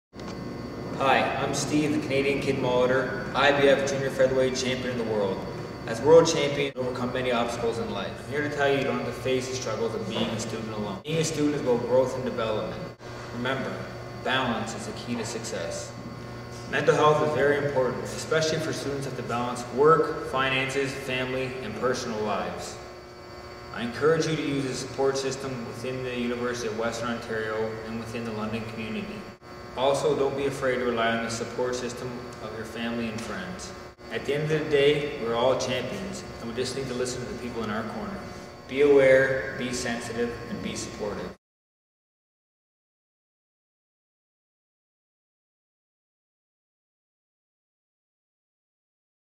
Special Message from Steve "The Canadian Kid" Molitor
As a part of the USC's Mental Health Awareness program at the University of Western Ontario, World Boxing Champion, Steve Molitor shares a special message to students about the importance of mental health.

for more information about the campaign!